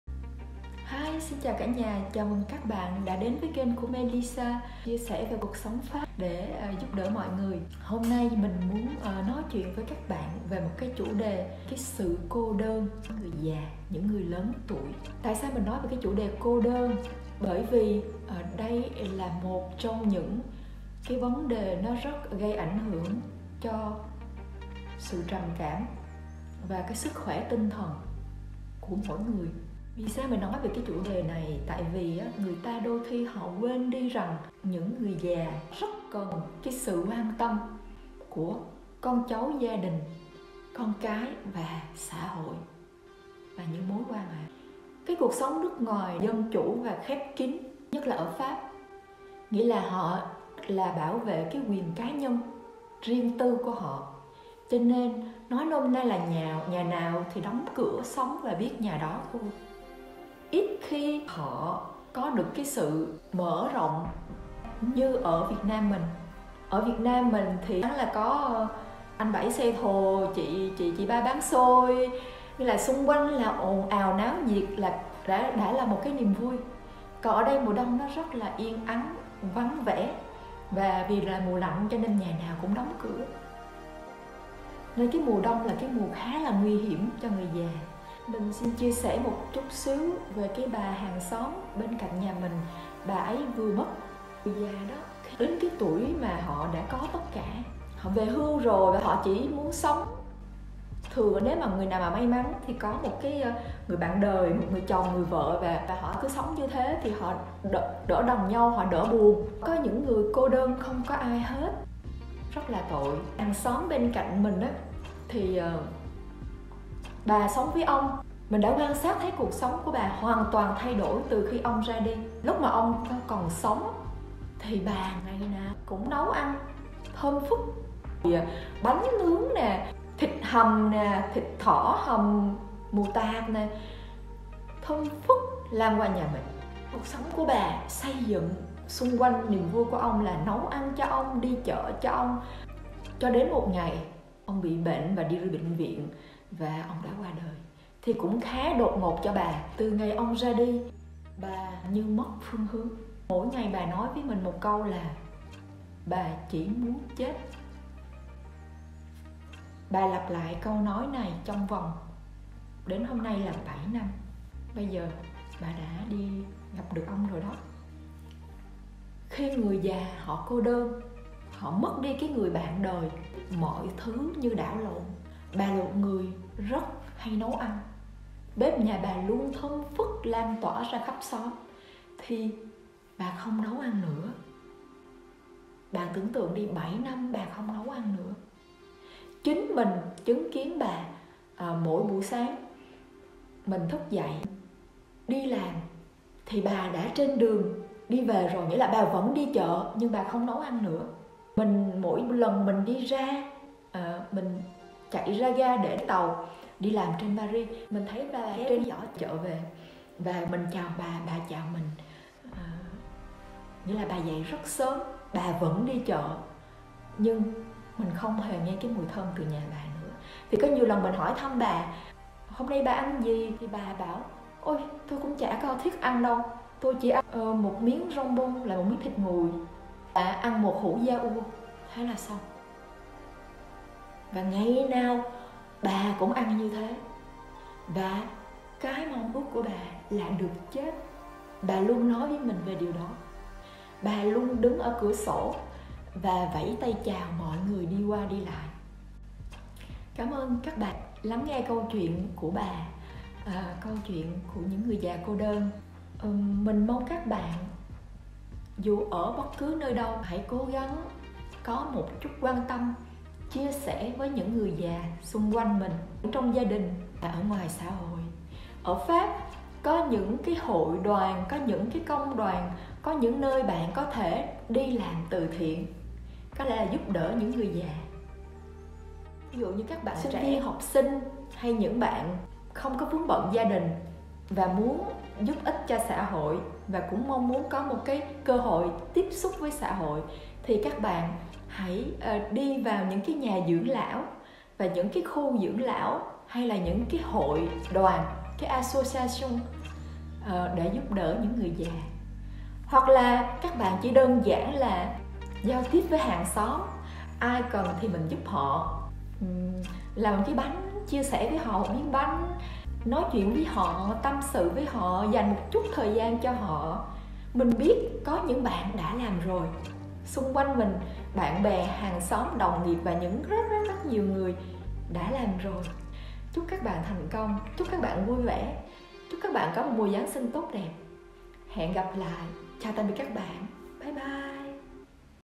CUỘC SỐNG PHÁP | CÔ ĐƠN TUỔI XẾ CHIỀU
Cuộc sống Pháp - CÔ ĐƠN GIỮA MÙA ĐÔNG GIÁ LẠNH?
Người già ở Pháp họ rất cô đơn khi mà con cái xa dần, tạo lập cuộc sống riêng của chúng.
SỰ CÔ ĐƠN, có thể giết chết tinh thần của bất kỳ ai, ở bất kỳ lứa tuổi nào.

Mùa đông lạnh ở châu âu có làm bạn cảm thấy cô đơn ?  cảm thấy trống vắng? cảm thấy chơi vơi?
Đó là những dấu hiệu của trầm cảm mùa đông.